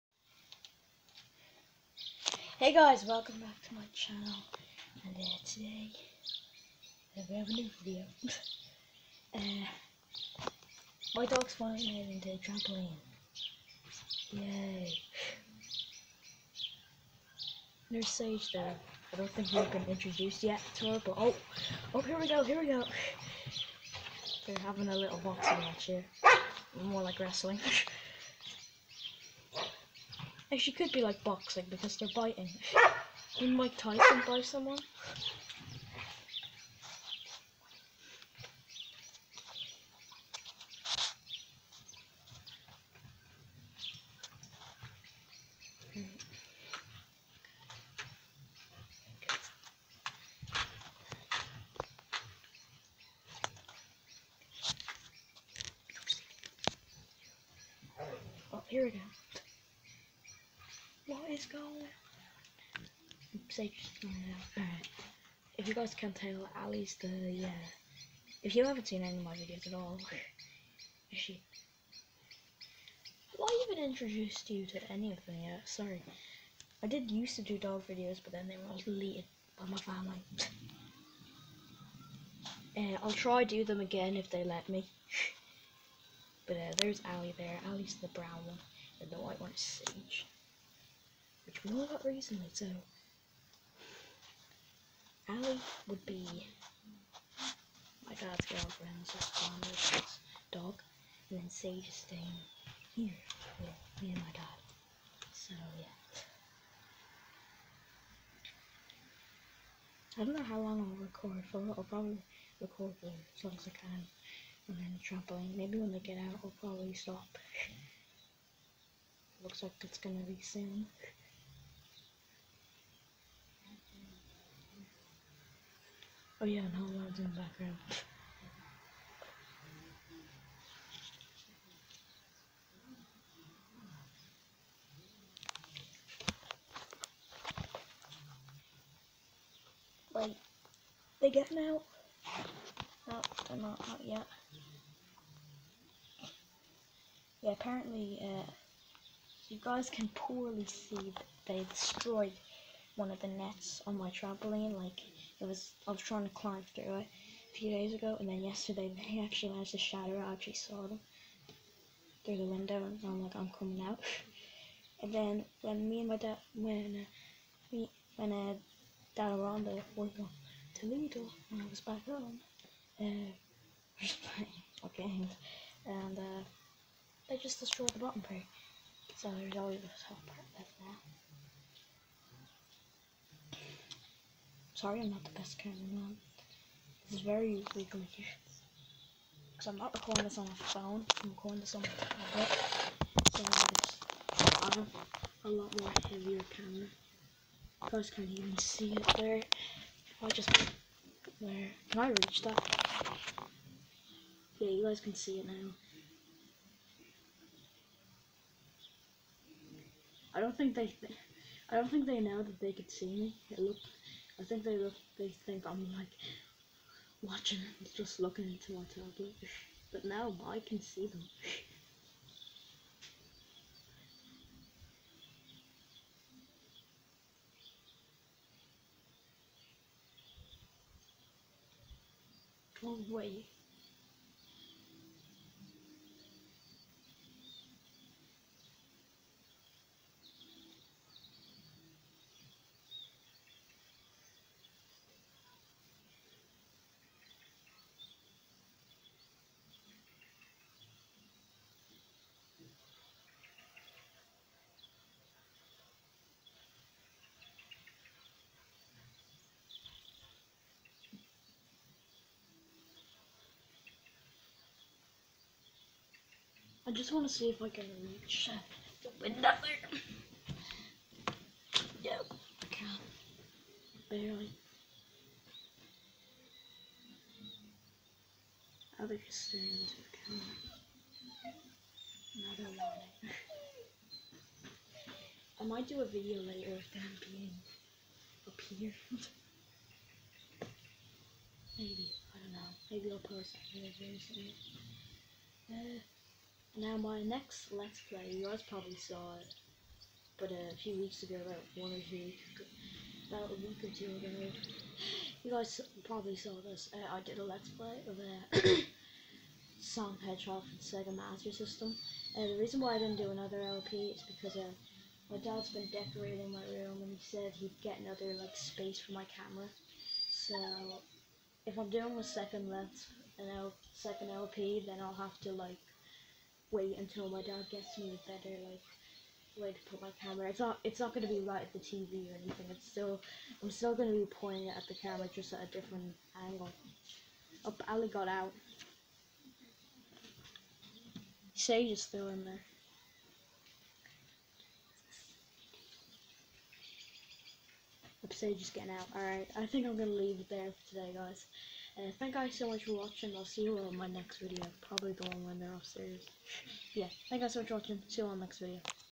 My two dogs,Allie and sage in the trampoline
Today instead of starting my second LP I have decided to do a video of Allie and sage my dogs in the trampoline make sure to subscribe we are only 1 sub away from 25 so make sure to sub like the video and hit the notification bell